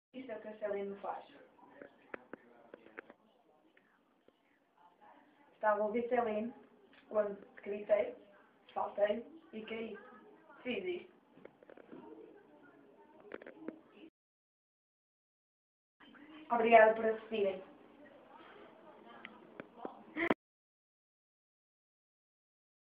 Celine Dion Fan Hurts herself because of celine dion
my cousin was hearing celine and she falls :D